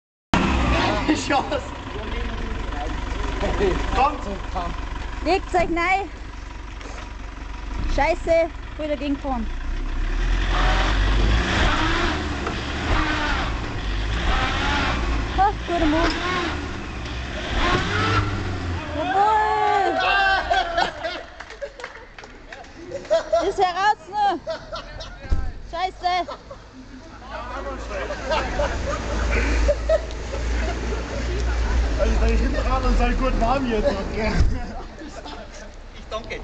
Taxi hängt an da Bank
Taxi hat einen kurzen Halt an einer bank unternommen, das ganze war auf da Wiesn 2006